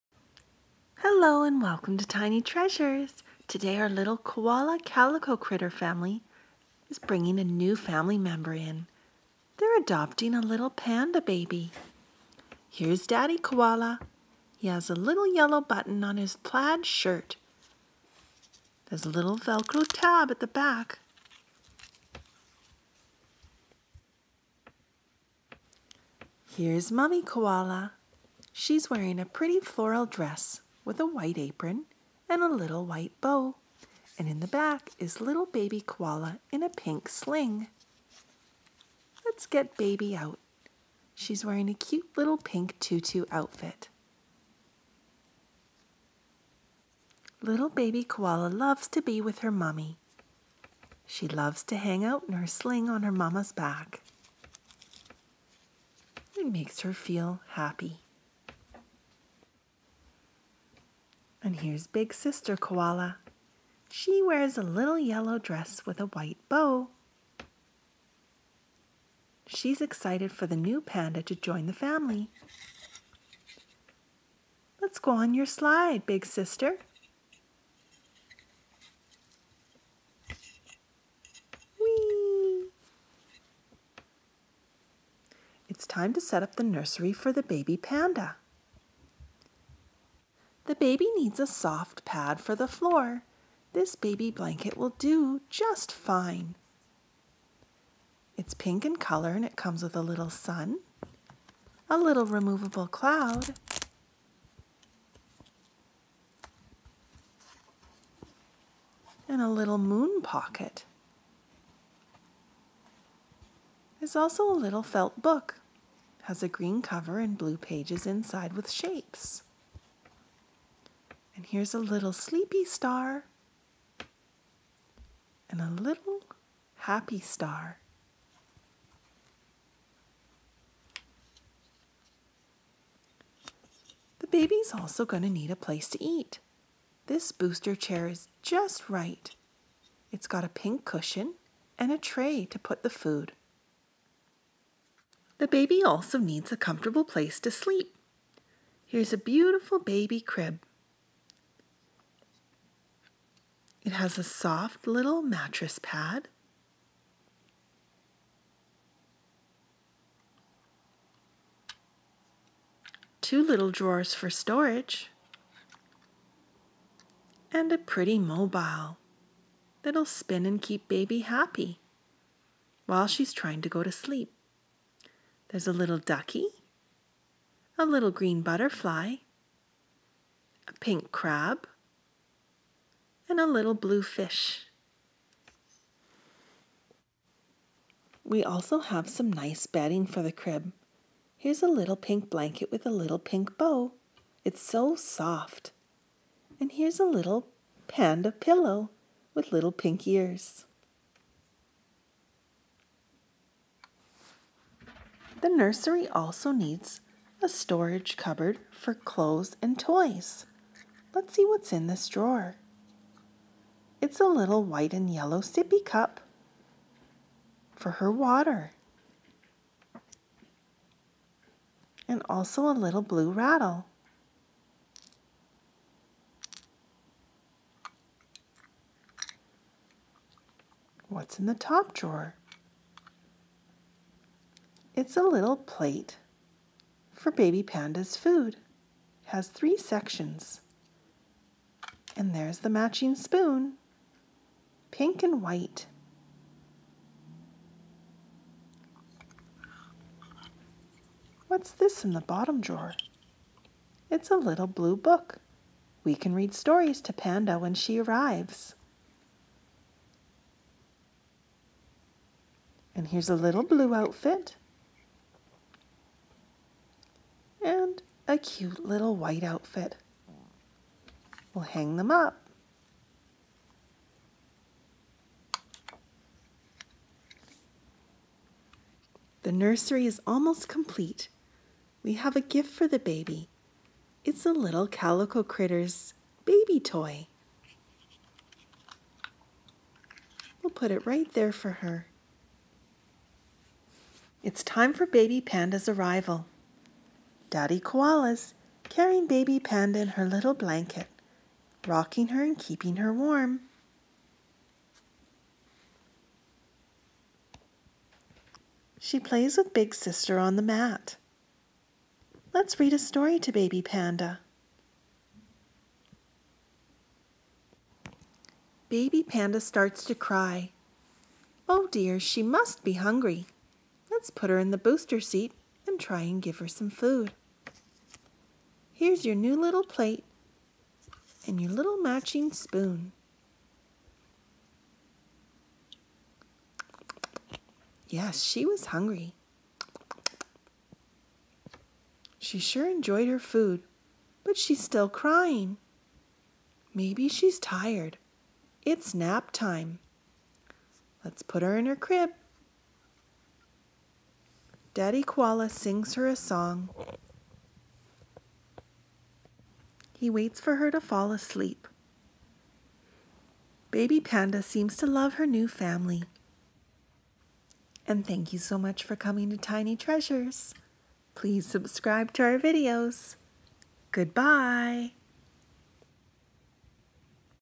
Koala CALICO CRITTERS Family Build Nursery for Baby Panda Toys!
Tiny Treasures builds a cute nursery for the baby Panda arriving into to join the Koala family. Enjoy!